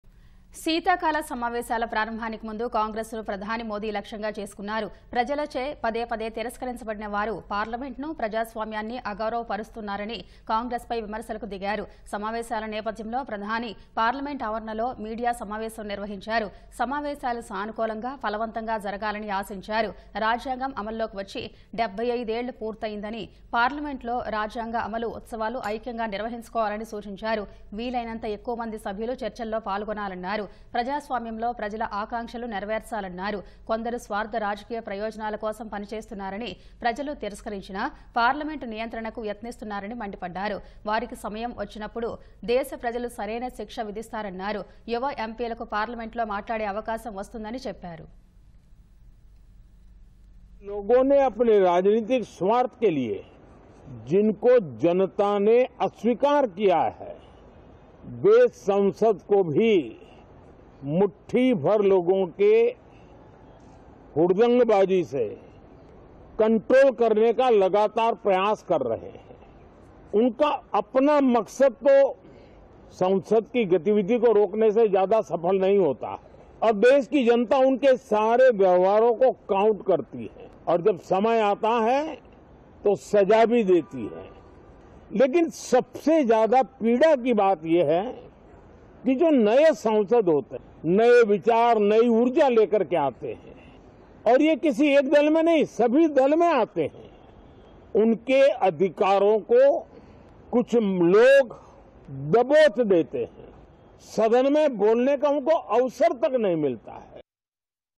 కాంగ్రెస్‌పై ప్రధాని మోదీ ఘాటు విమర్శలు | PM Modi Scothing Attack On Cong | Ahead Of Winter Sessions
శీతాకాల సమావేశాల ప్రారంభానికి ముందు కాంగ్రెస్ ను ప్రధాని మోదీ లక్ష్యంగా చేసుకున్నారు. ప్రజలచే పదేపదే తిరస్కరించబడిన వారు పార్లమెంటును, ప్రజాస్వామ్యాన్ని అగౌరవపరుస్తారని కాంగ్రెస్ పై విమర్శలకు దిగారు. సమావేశాల నేపథ్యంలో.. ప్రధాని పార్లమెంటు ఆవరణలో మీడియా సమావేశం నిర్వహించారు. సమావేశాలు సానుకూలంగా, ఫలవంతంగా జరగాలని ఆశించారు. రాజ్యాంగం అమల్లోకి వచ్చి 75 ఏళ్లు పూర్తయిందని, పార్లమెంటులో రాజ్యాంగ అమలు ఉత్సవాలు ఐక్యంగా నిర్వహించుకోవాలని సూచించారు. వీలైనంత ఎక్కువ మంది సభ్యులు చర్చల్లో పాల్గొనాలన్నారు. ప్రజాస్వామ్యంలో ప్రజల ఆకాంక్షలు నెరవేర్చాలన్నారు. కొందరు స్వార్థ రాజకీయ ప్రయోజనాల కోసం పనిచేస్తున్నారని, ప్రజలు తిరస్కరించినా పార్లమెంటు నియంత్రణకు యత్నిస్తన్నారని మండిపడ్డారు. వారికి సమయం వచ్చినప్పుడు దేశ ప్రజలు సరైన శిక్ష విధిస్తారన్నారు. యువ ఎంపీలకు పార్లమెంటులో మాట్లాడే అవకాశం వస్తుందని చెప్పారు.